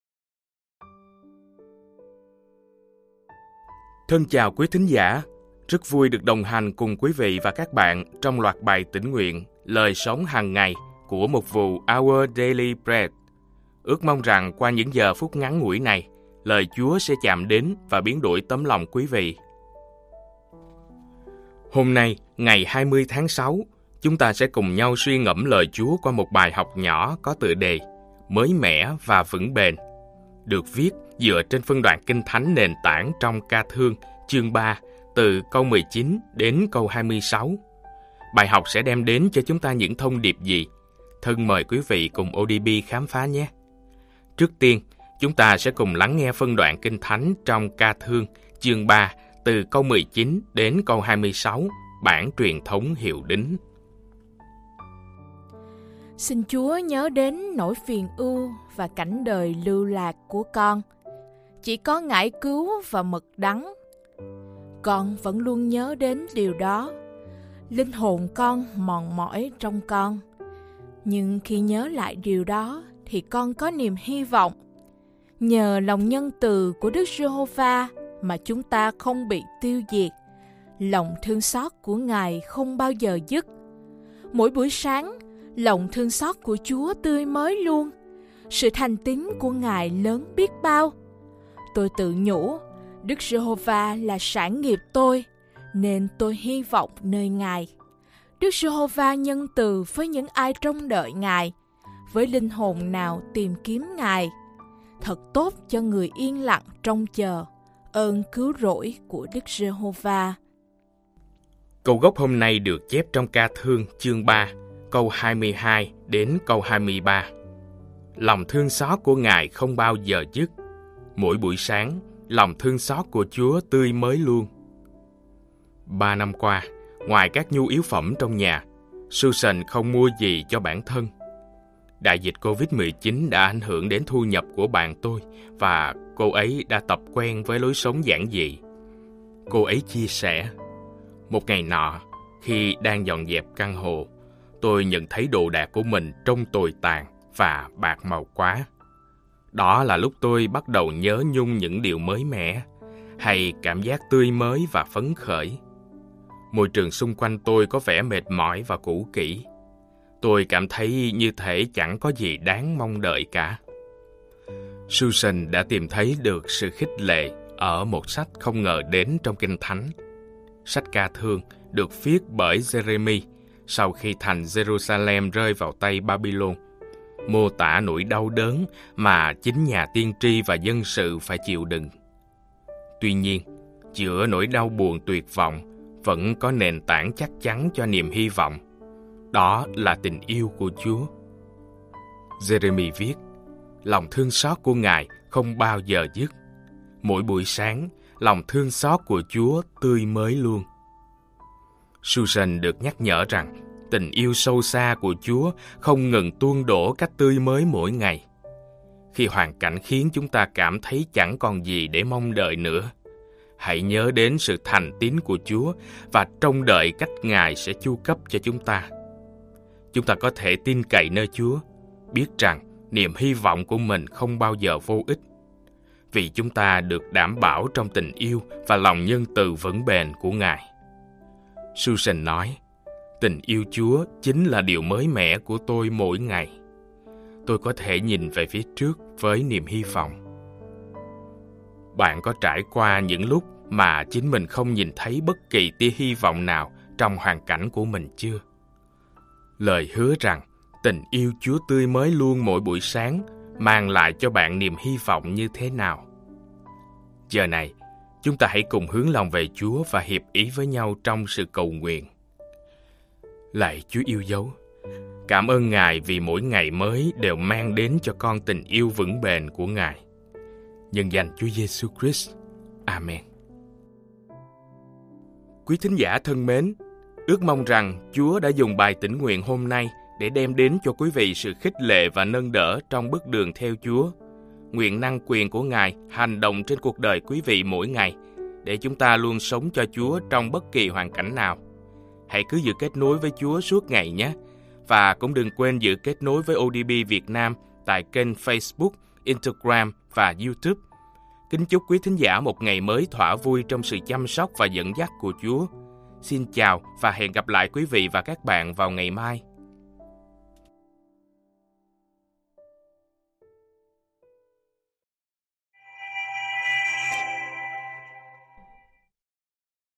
Mới Mẻ Và Vững Bền | Tĩnh nguyện Lời Sống Hằng Ngày | 20 Tháng 06, 2024
Ba năm qua, ngoài các nhu yếu phẩm trong nhà, Susan không mua gì cho bản thân. Đại dịch Covid-19 đã ảnh hưởng đến thu nhập của bạn tôi và cô ấy đã tập quen với lối sống giản dị. Cô chia sẻ: “Một ngày nọ, khi đang dọn dẹp căn hộ, tôi nhận thấy đồ đạc của mình trông tồi tàn và bạc màu quá. Đó là lúc tôi bắt đầu nhớ nhung những điều mới mẻ, hay cảm giác tươi mới và phấn khởi. Môi trường xung quanh tôi có vẻ mệt mỏi và cũ kỹ. Tôi cảm thấy như thể chẳng có gì đáng mong đợi cả”.

Susan đã tìm thấy sự khích lệ ở một sách không ngờ đến trong Kinh Thánh. Sách Ca Thương được viết bởi Giê-rê-mi sau khi thành Giê-ru-sa-lem rơi vào tay Ba-by-lôn, mô tả nỗi đau đớn mà chính nhà tiên tri và dân sự phải chịu đựng. Tuy nhiên, giữa nỗi đau buồn tuyệt vọng vẫn có nền tảng chắc chắn cho niềm hy vọng, đó là tình yêu của Chúa. Giê-rê-mi viết: “Lòng thương xót của Ngài không bao giờ dứt. Mỗi buổi sáng, lòng thương xót của Chúa tươi mới luôn” (3:22-23).

Susan được nhắc nhở rằng tình yêu sâu xa của Chúa không ngừng tuôn đổ cách tươi mới mỗi ngày. Khi hoàn cảnh khiến chúng ta cảm thấy chẳng còn gì để mong đợi nữa, hãy nhớ đến sự thành tín của Chúa và trông đợi cách Ngài sẽ chu cấp cho chúng ta. Chúng ta có thể tin cậy nơi Chúa, biết rằng niềm hy vọng của mình không bao giờ vô ích (c.24-25) vì chúng ta được bảo đảm trong tình yêu và lòng nhân từ vững bền của Ngài.

Susan nói: “Tình yêu Chúa chính là ‘điều mới mẻ’ của tôi mỗi ngày. “Tôi có thể nhìn về phía trước với niềm hy vọng”.

Bạn có từng trải qua những lúc mà chính mình không nhìn thấy bất kỳ tia hy vọng nào trong hoàn cảnh của mình chưa? Lời hứa rằng tình yêu Chúa “tươi mới luôn” mỗi buổi sáng mang lại cho bạn niềm hy vọng như thế nào?

Lạy Chúa yêu dấu, cảm ơn Ngài vì mỗi ngày mới đều mang đến cho con tình yêu vững bền của Ngài.

Chú giải
Ca Thương là một trong những sách đau buồn nhất của Kinh Thánh; đó là tiếng than khóc từ tấm lòng của Giê-rê-mi dành cho dân tộc của ông đang bị thương tích. Nhưng tại đây, ngay giữa bài thơ của ông, chúng ta tìm thấy niềm hy vọng thực sự: “Lòng thương xót của Ngài không bao giờ dứt” (3:22). Sách Ca Thương cũng kết thúc trong niềm hy vọng, tuy nhiên chúng ta vẫn cảm nhận sự nghi ngờ của Giê-rê-mi. Ông tuyên bố: “Lạy Đức Giê-hô-va, Ngài trị vì đời đời, ngôi Ngài tồn tại từ thế hệ nầy sang thế hệ kia” (5:19). Đó là cách kết thúc mà chúng ta mong đợi – trong chiến thắng tối thượng. Tuy nhiên, vị tiên tri này đã hỏi Chúa hai câu hỏi day dứt: “Sao Ngài quên chúng con mãi mãi, lìa bỏ chúng con lâu ngày như vậy?” (c.20). Mặc dù ông cầu xin Chúa “phục hồi chúng con” (c.21), nhưng ông không cầu xin với lòng tin vững chắc, mà nói thêm rằng: “Phải chăng Ngài đã cương quyết từ bỏ chúng con, và giận chúng con vô cùng?” (c.22). Tiên tri Giê-rê-mi đã tranh chiến rất nhiều với những đau thương mà ông chứng kiến.